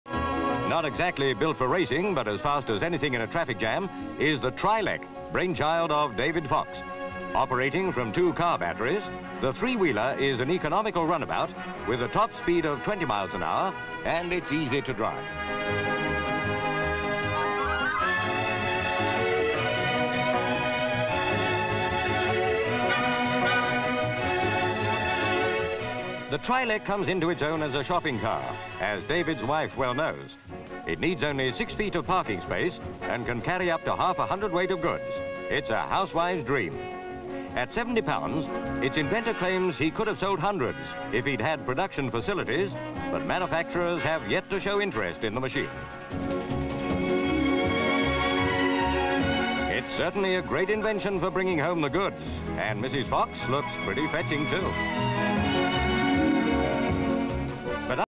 New Form Of Transport - The Trilec (1966)
Kent.

SV. Of the new electric three wheel shopping car, the inventor lifts up the seat. CU. Seat being lifted up. CU. The inventor's hand making adjustments under seat to the battery. CU. The engine. SV. Inventor closes the seat down and gets on. CU. Hand switching on the engine. CU. The inventor. CU. Handle bars, hand twisting the twist grip. SV. As the electric car drives away. SV. Of the electric car parked outside shop. Woman, wife of the inventor, comes out with two shopping baskets and places them on carrier at the front. SV. As the woman drives off. Travelling shot, woman driving along. CU. Travelling shot, woman driving, & CU. SV. Travelling shot, woman driving along in electric car.
 FILM ID:2021.3